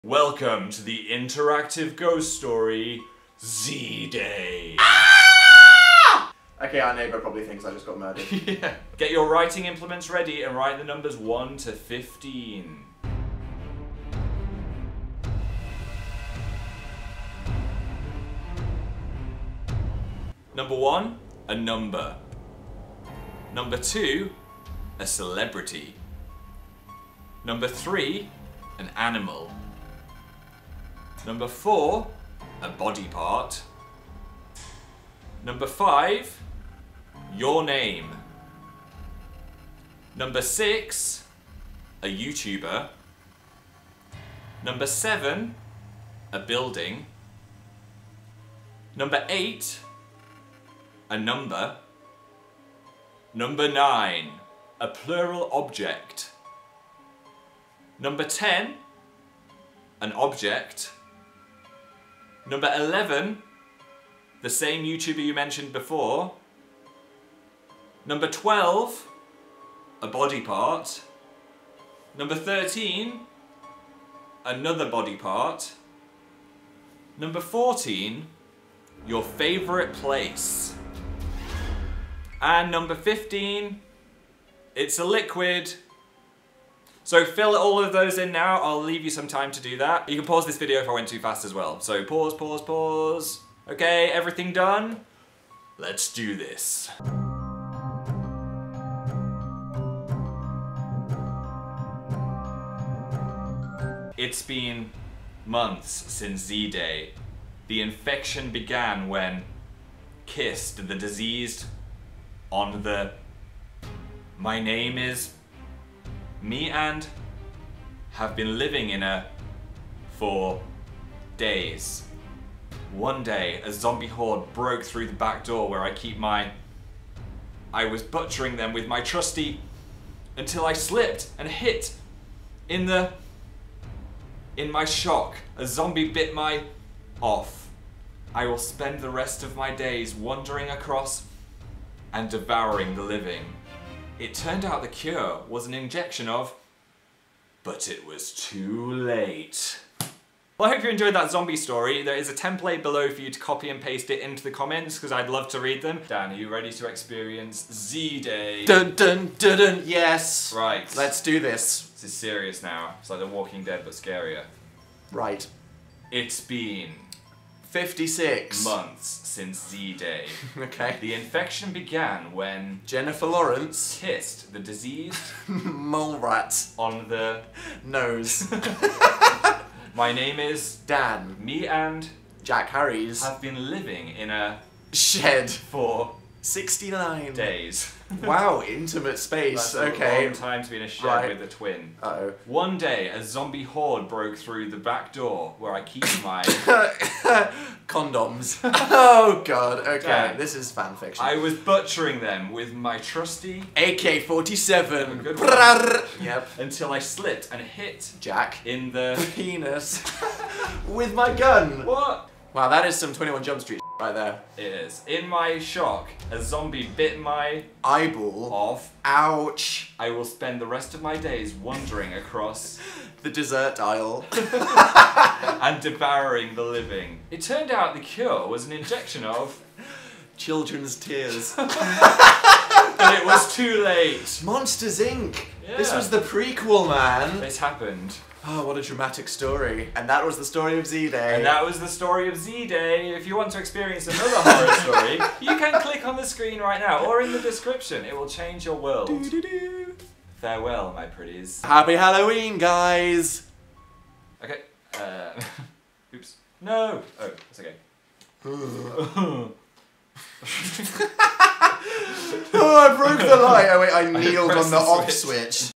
Z - DAY!
Create your own Zombie story!

COPY AND PASTE YOUR STORY IN THE COMMENTS :D

it's been ____ months since z-day. The infection began when _______ kissed the diseased ______ on the _______. my name is ____. me and _____ have been living in a _____ for ____ days. one day a zombies broke in where i keep my ____s. i butchered them with my trusty ____ until i slipped and hit ______ in the _____. in my shock a zombie bit my _____ off. i remain in ____ devouring the living. it turned out the cure was an injection of ____. but it was TOO LATE.